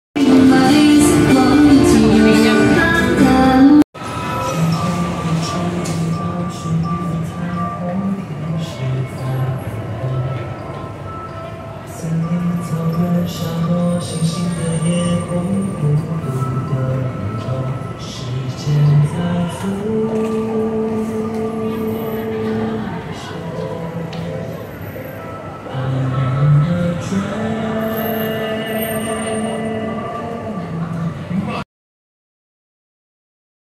20230512 MRT蘇坤逸站
MRT蘇坤逸站平面入口是二張照片的應援，20分鐘一次（站在警衛前），還有一帳是卡通版沒等，和往BTS的轉彎處的曲牆螢幕，站位不敢撐在人家小攤前，
拍的當到是下班四五點時人潮多，
所以只拍一次就趕時間走了。